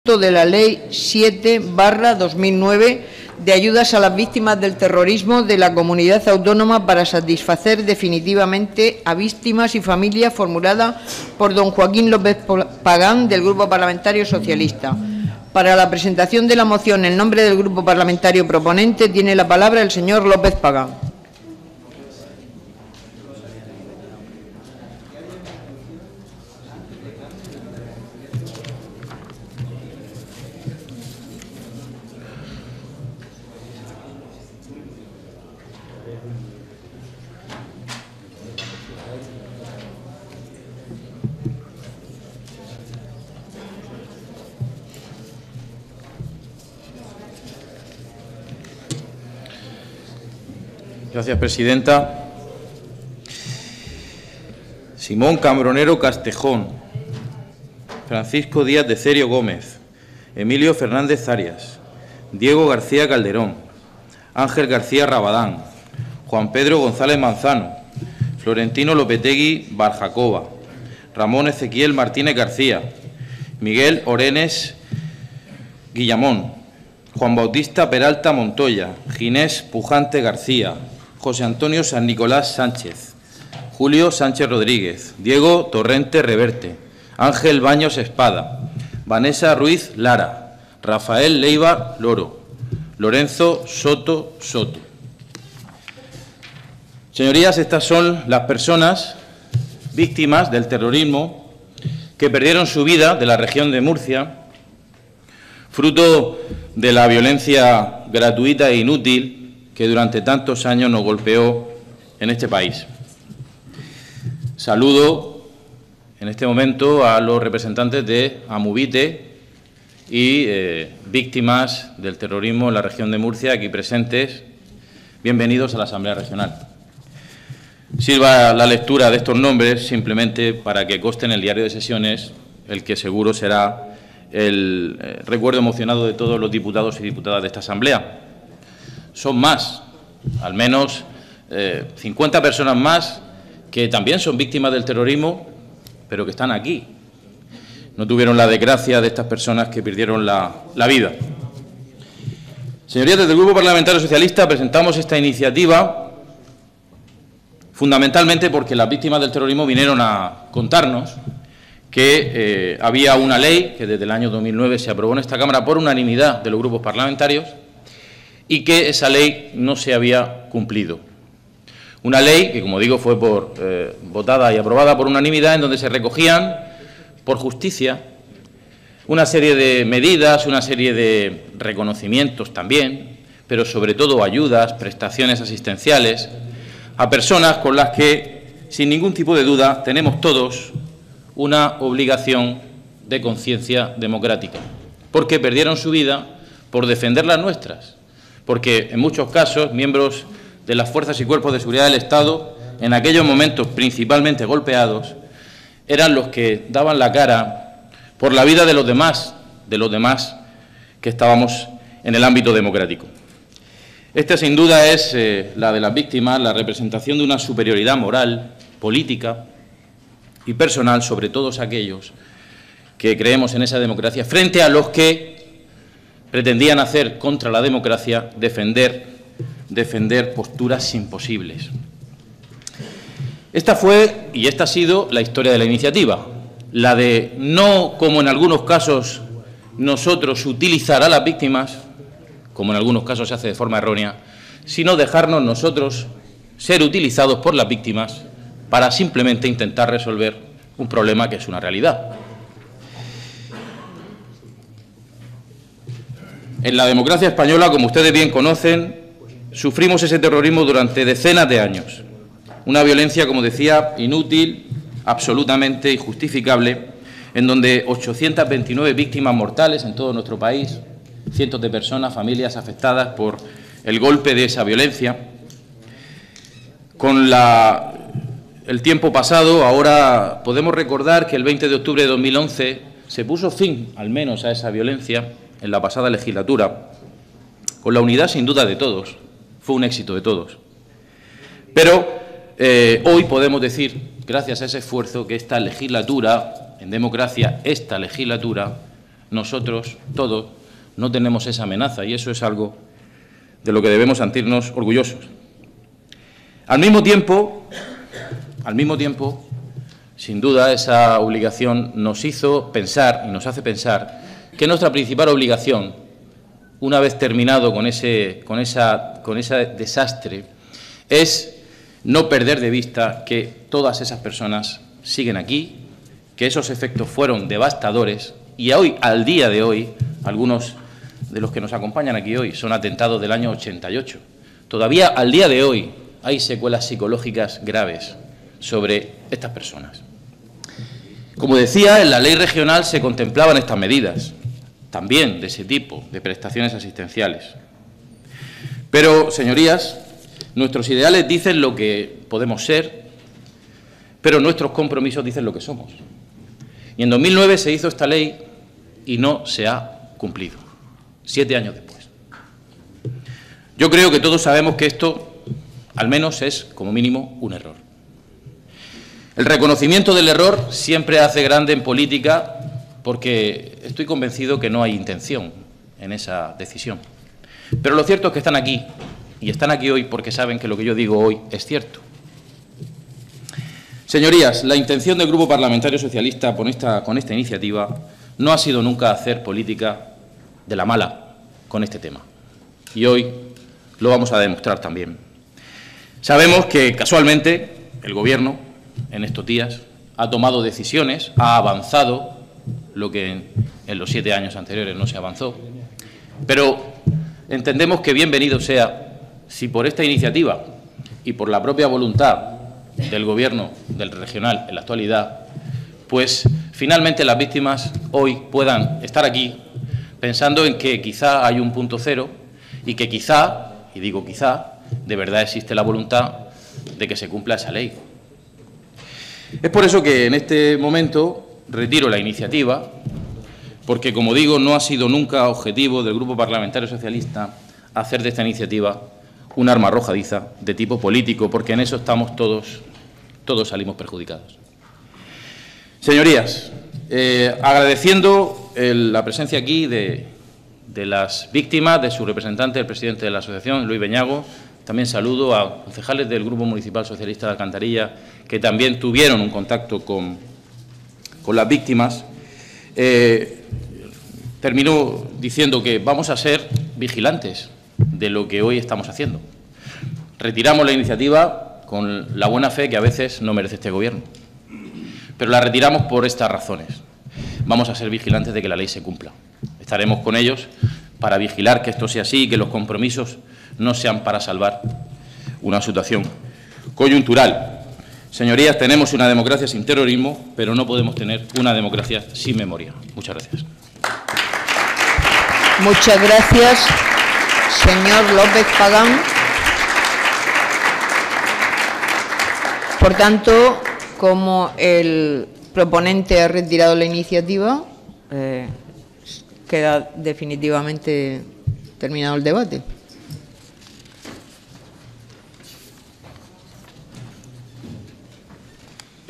9L MOCP 0563
MOCIÓN SOBRE CUMPLIMIENTO DE LA LEY 7/2009 DE AYUDAS A LAS VÍCTIMAS DEL TERRORISMO DE LA COMUNIDAD AUTÓNOMA PARA SATISFACER DEFINITIVAMENTE A  VÍCTIMAS Y FAMILIAS, FORMULADA POR D. JOAQUÍN LÓPEZ PAGÁN, DEL G.P. SOCIALISTA.